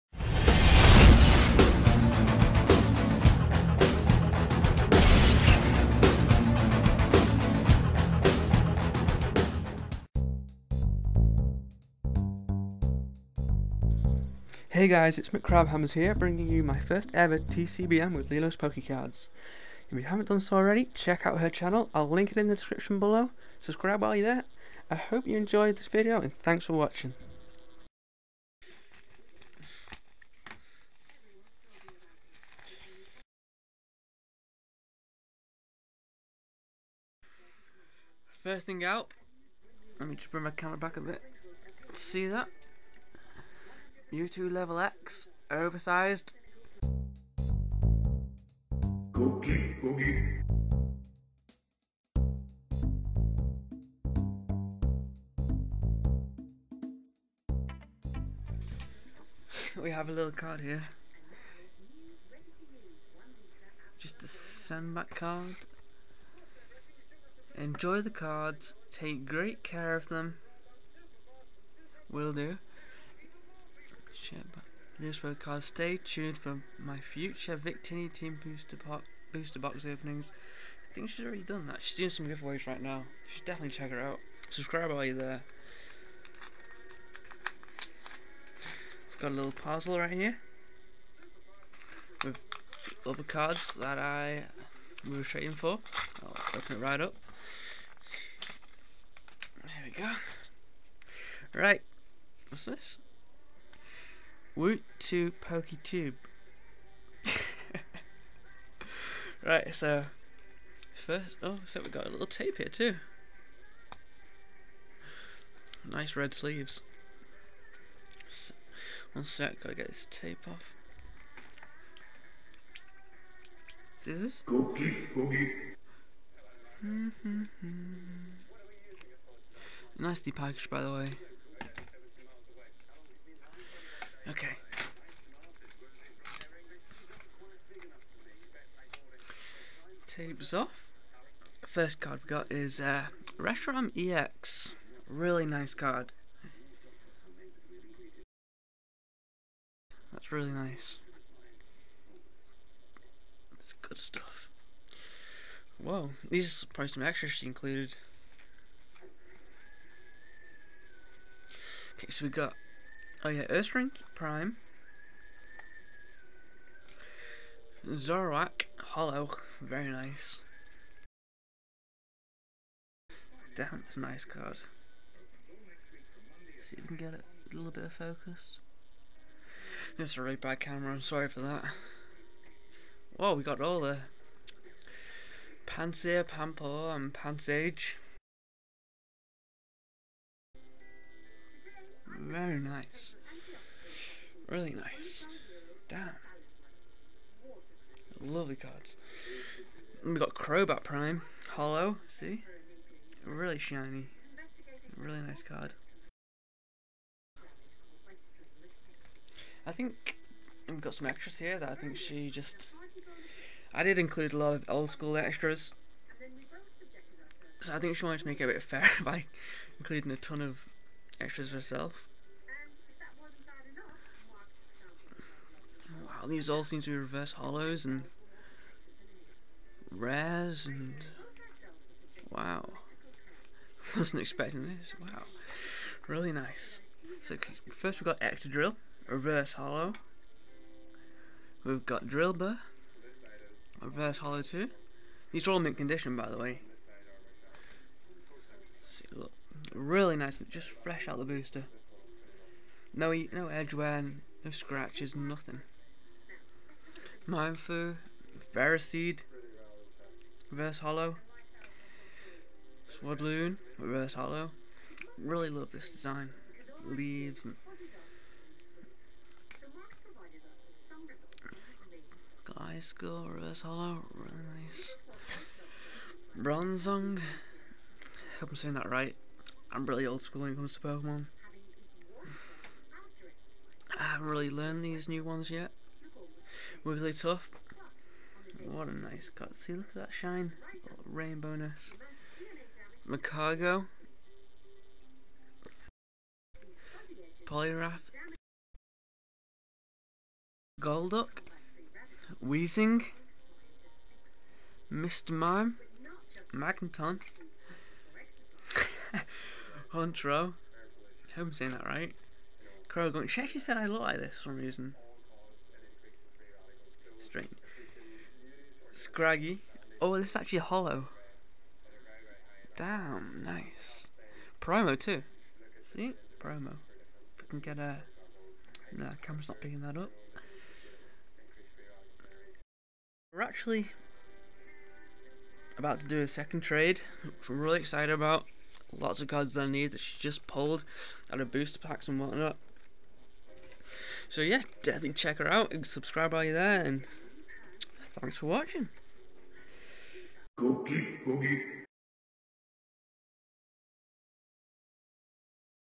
TCBM #1 - Lilospokecards | Super Huge Sparkly Card?! O_o
My first ever TCBM!! and first video with talking! (Shy) let me know what you think! :D
Super magical sparkly cards, gem mint condition!!! great trade!